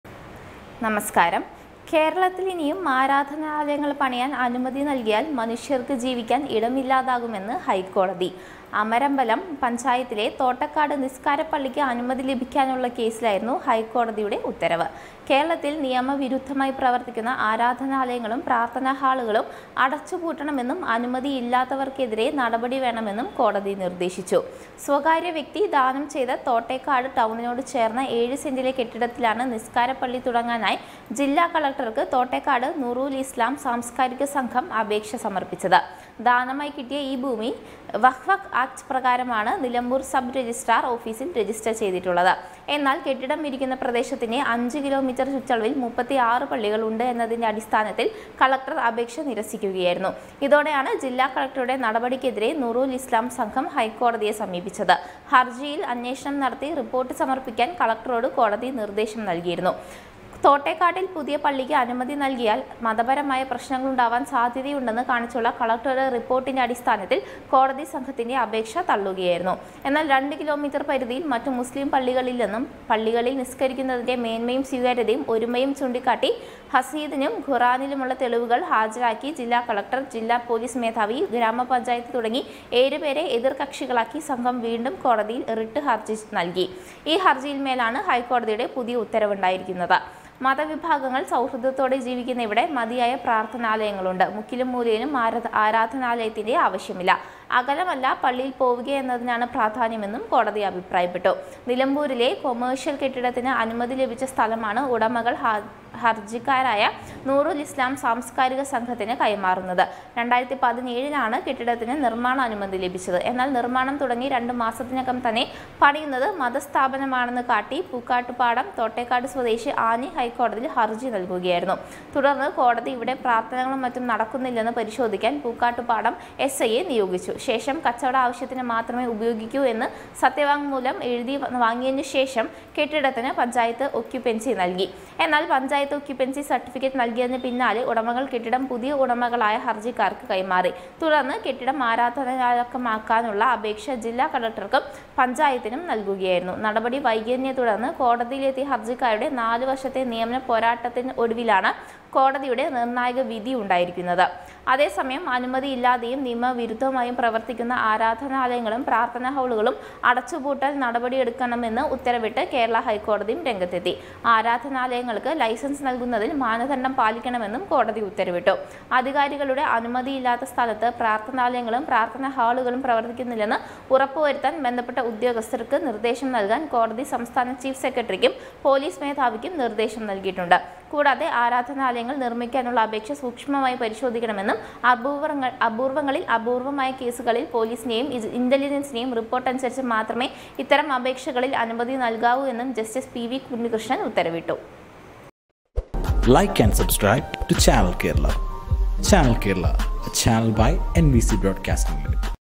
തോട്ടക്കാട് പള്ളിക്ക് അനുമതിയില്ല ; നടപടി വേണമെന്ന് കോടതി
ഫേസ്ബുക്ക് പേജ് പിന്തുടരാന്‍ ലിങ്ക് ഓപ്പണ്‍ ചെയ്ത് ലൈക്ക് ചെയ്യുക.